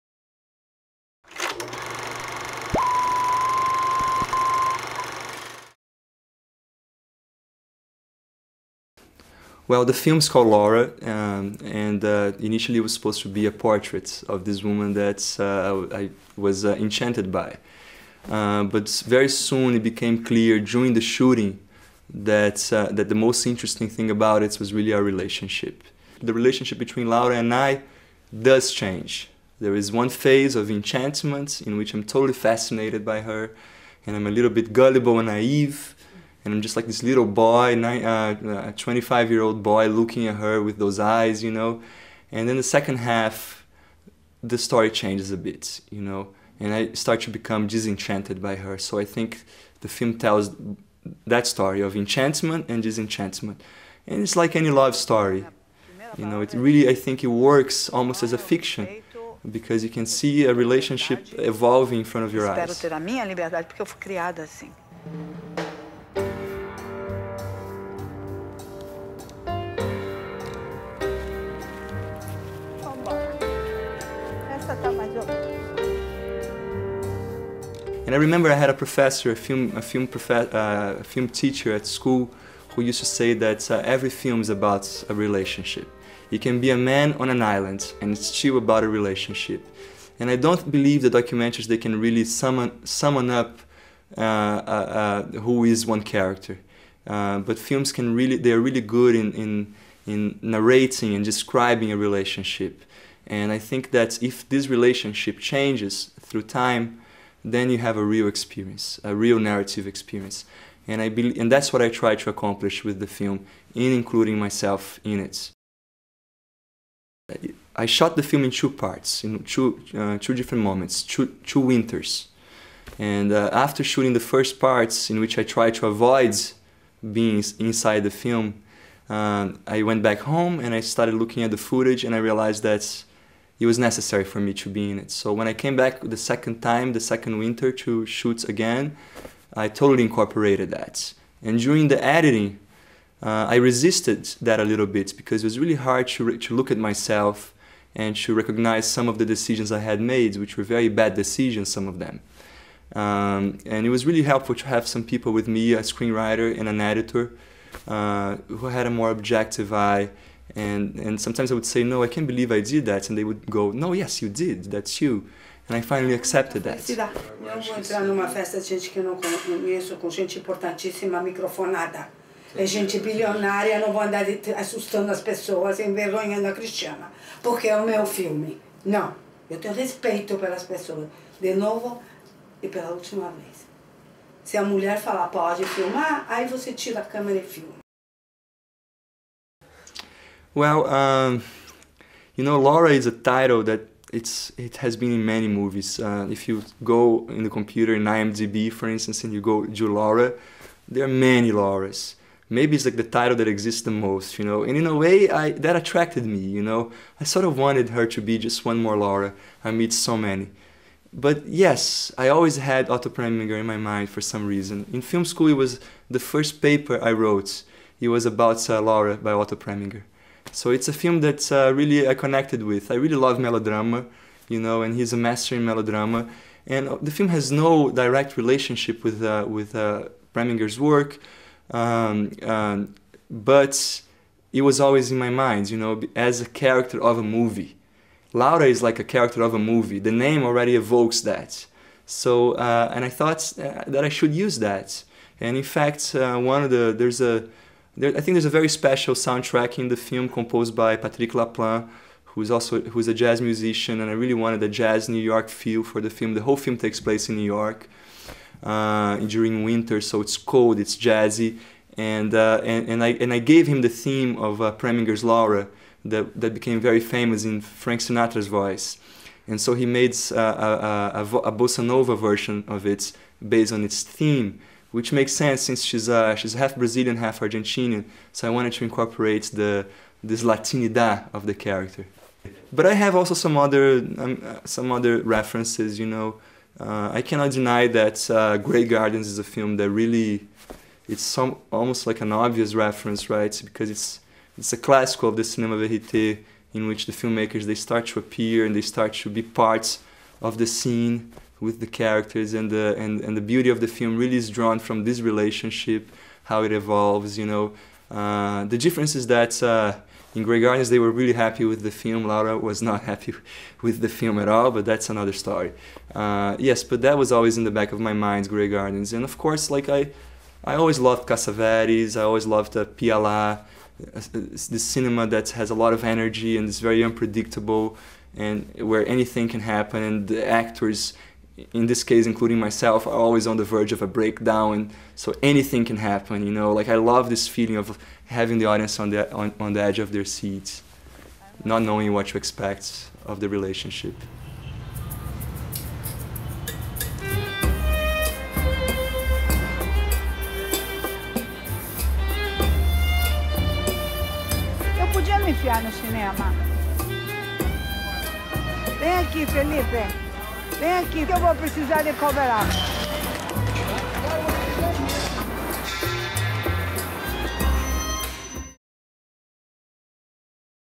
Fellipe Barbosa's Interview | Laura
Laura is a creature of the night. She loves the flash and glamour of the star system. Night after night she is out meeting Hollywood's heavy hitters. But this is only the surface. The truth is harsher. Of Brazilian descent, Laura has hit New York hoping to make it big time. Living in a tiny flat, crowded with boxes and plastic bags, she treads a very thin line between flash and extreme poverty.

Video by:
Filmmakers: Arthur Touchais, Myriam Rachmuth, Josua Hotz, Léo Haddad, Malika Pellicioli, Mélanie Rappo, Filippo Demarchi, Julia Sagnakkara, Gregory Casares
Teacher: Benoît Rossel
ECAL / University of art and design Lausanne